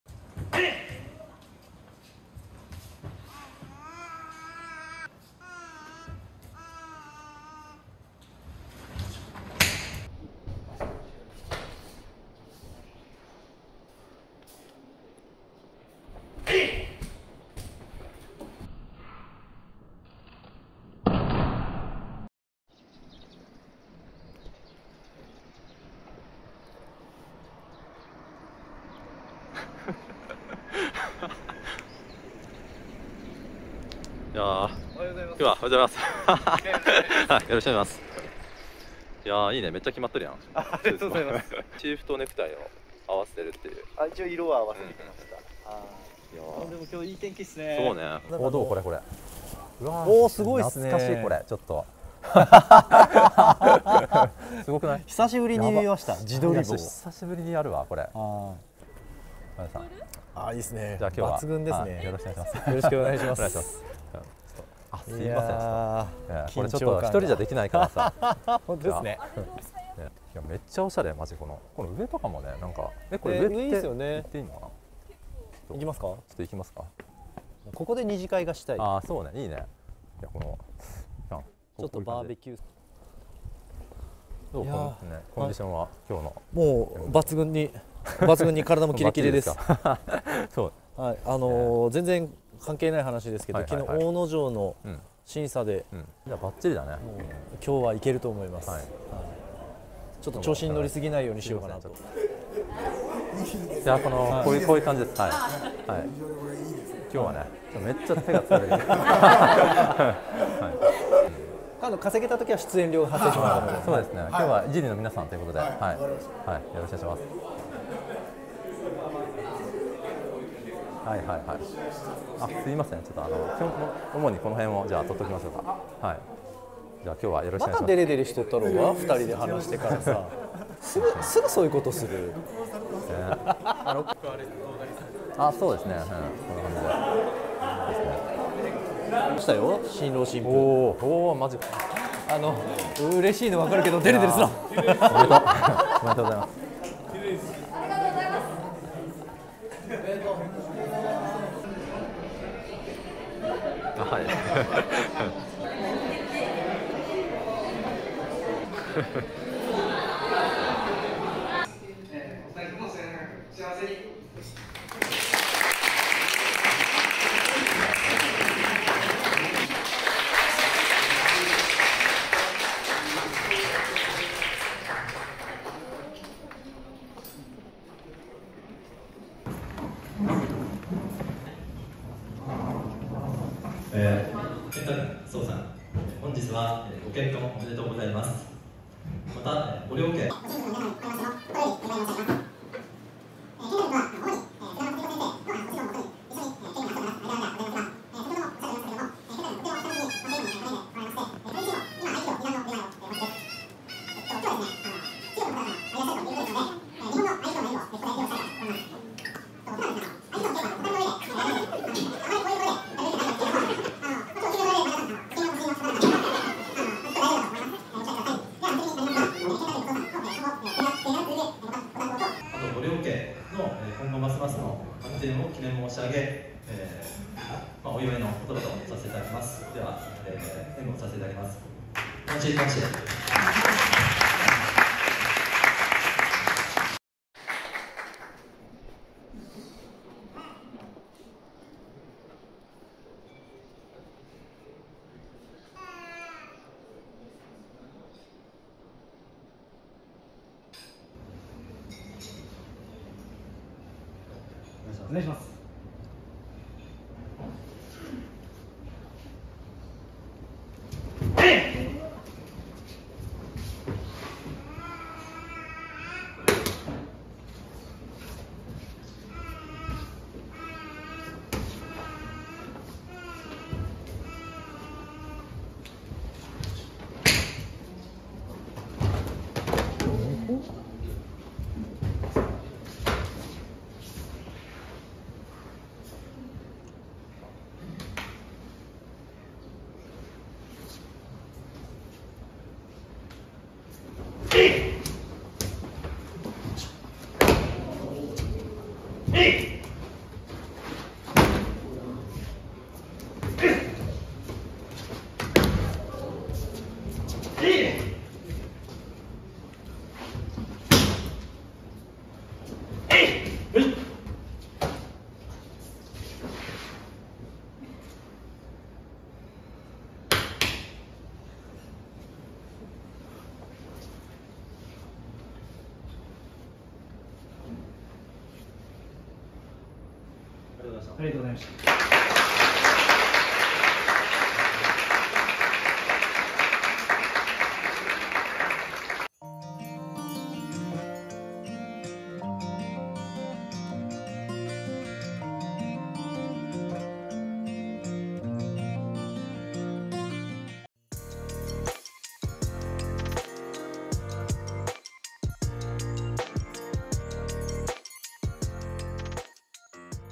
結婚式の余興で合気道の演武披露してみた
道友の上西さんが結婚されたので披露宴で合気道の演武をしてきました。
動画の始めはフリートークなので飽きたら4:00あたりまで飛ばしてください。
0:00 オープニング
0:22 博多の森
1:42 元井尻道場の方々
3:00 結婚式スピーチ（２倍速）
3:57 合気道演武
5:39 エンディング